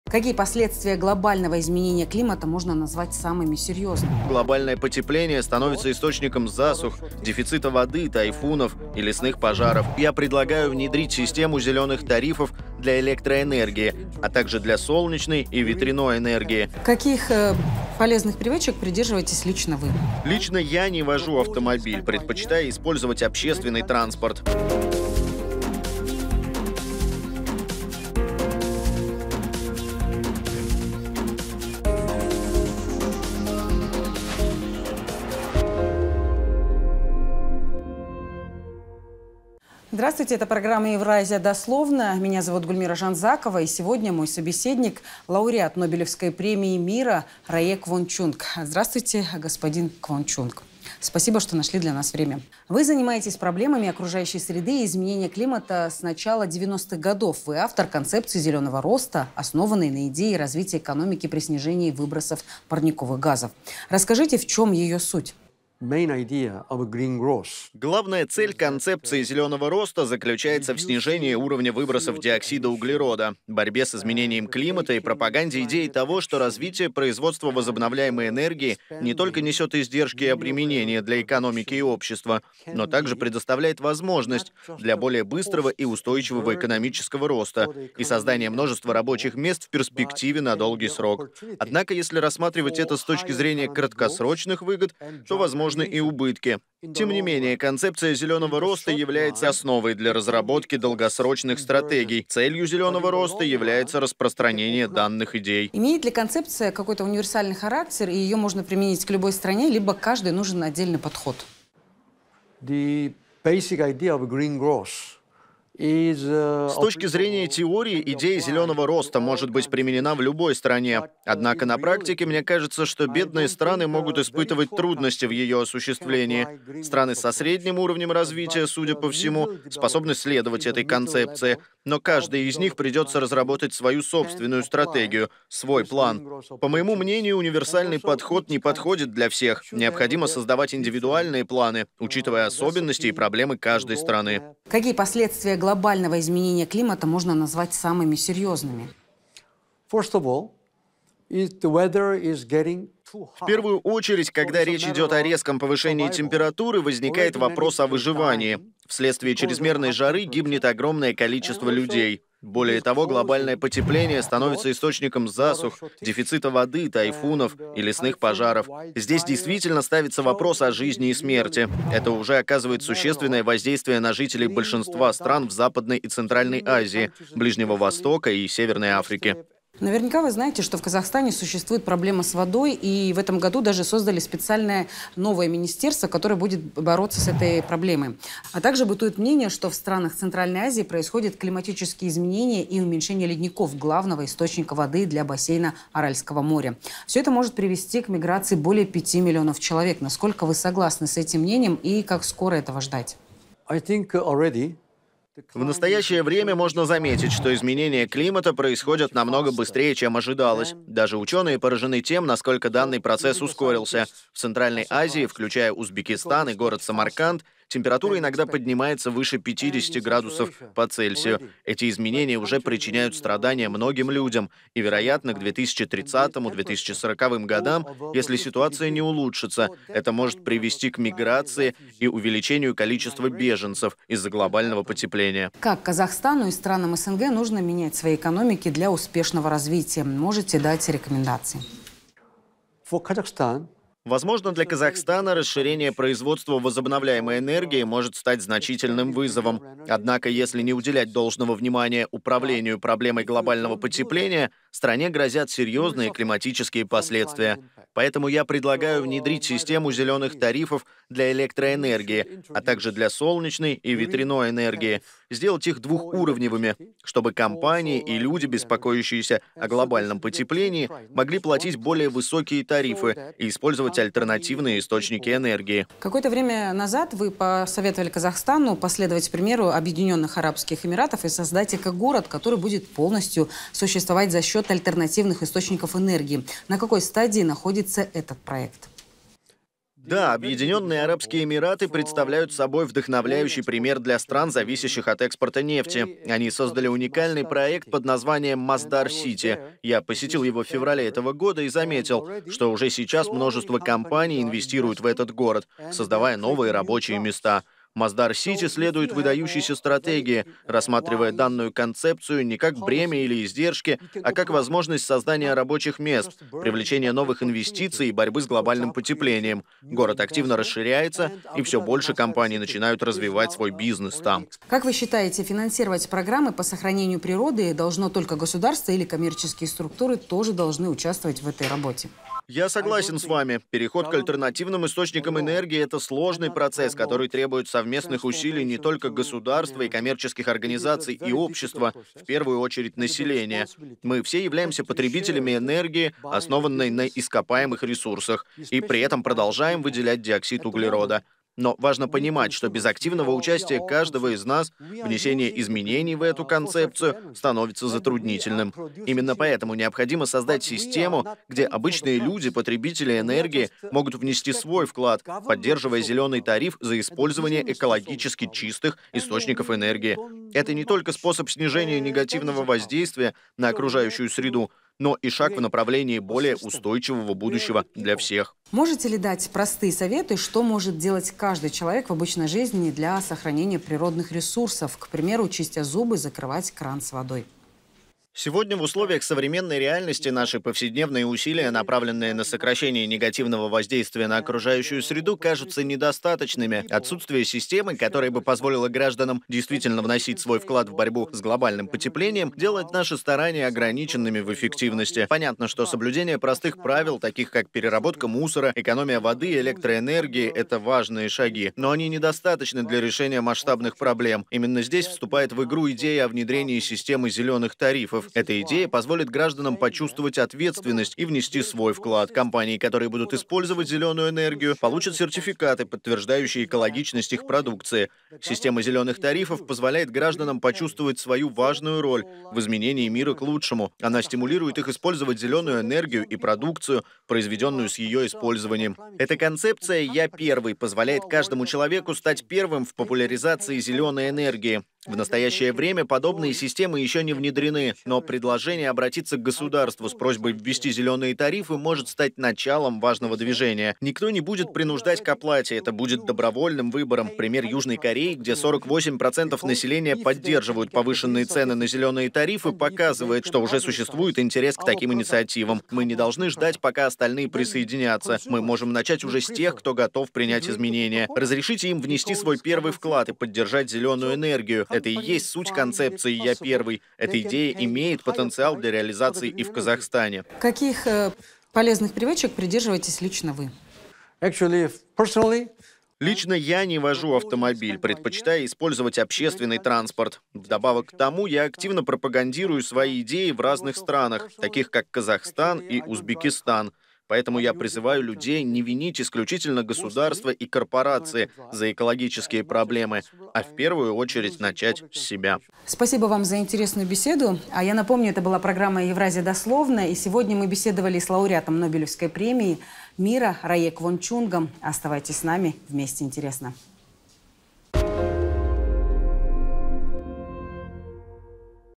Нобелевский лауреат Рае Квон Чунг о страшных последствиях глобального изменения климата в мире
Лауреат Нобелевской премии мира 2007 года Рае Квон Чунг занимается проблемами окружающей среды и вопросами изменения климата с начала 1990-х годов. Он автор концепции «зеленого роста», основанной на идее развития экономики при снижении выбросов парниковых газов. Корреспондент «МИР 24» Гульмира Жанзакова встретилась с экспертом и обсудила современные климатические изменения.